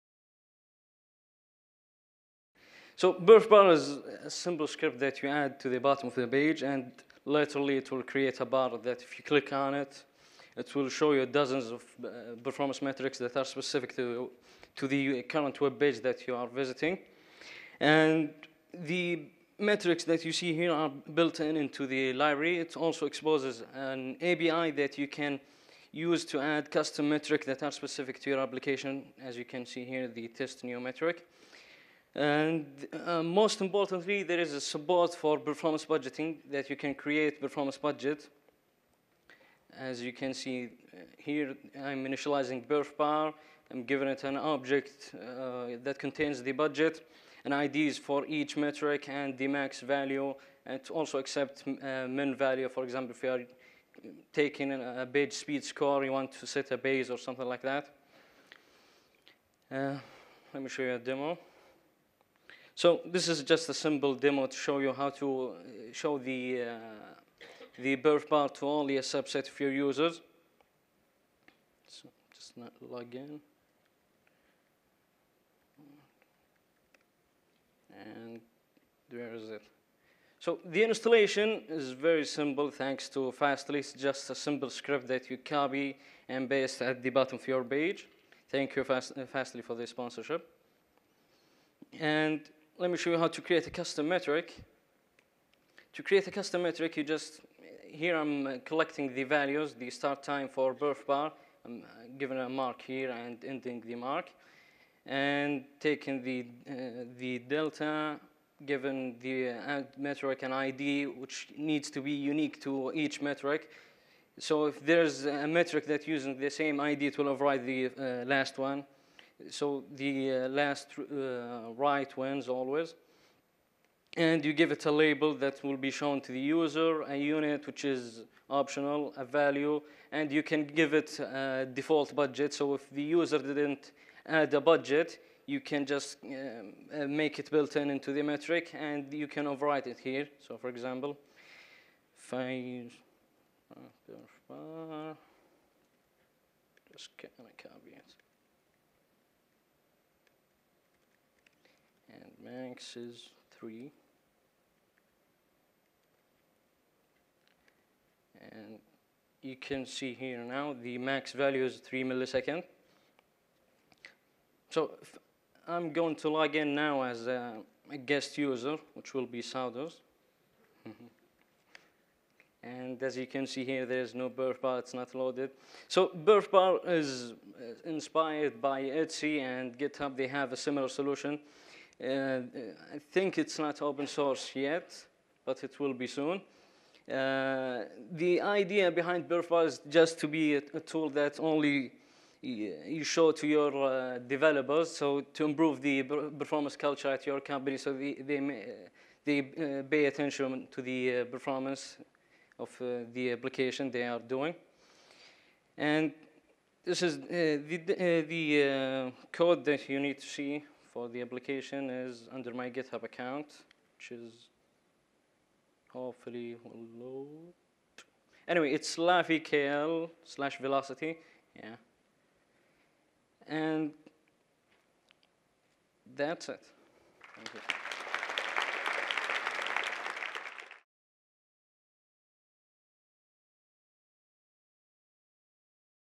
Khalid Lafi's PerfBar Lightning Demo - Velocity Europe 2014
A PerfBar lightning demo from Velocity Europe 2014 in Barcelona, "Always Keep an Eye on Your Website Performance - PerfBar."

In this lightning demo I will show you how to install and deploy PerfBar, walk you through the metrics, as well as show you how to create your own metrics.

I will go over how to leverage PerfBar to improve the performance of your website, and show you how it can help you improve the web performance culture at your company.

About Khalid Lafi (WireFilter):
A 21-year-old, back-end and front-end engineer from Saudi Arabia, Khalid’s day job is a Full-Stack engineer at WireFilter working on the customer-facing web tools that they provide for their customers. More than 80% of the internet traffic in the region goes through their machines, efficiently handling billions of requests daily.

At night, Khalid works at Crafted-Apps (his own startup) building products, or working with clients to improve their web apps performance and architecture, and transforming their ideas from concepts to a reality.

He also enjoys building open-source projects that help developers build faster web apps, such as Steady.js, PerfBar, and other projects on Github.